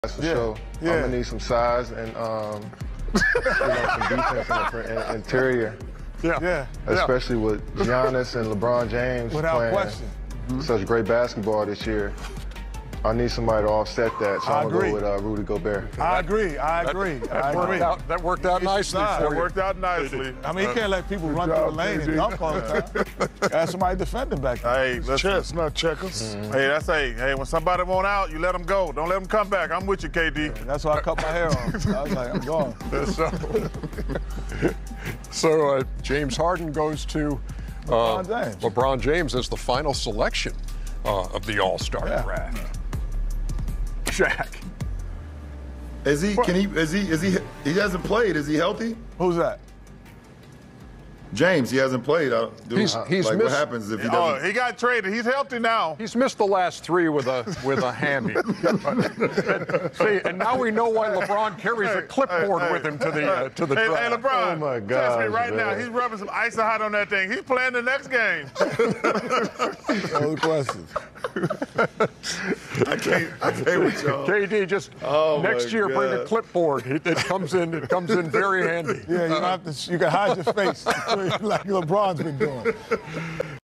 # KD Picking Gobert over Harden 😂😂🤪😜
2022 NBA All star draft pick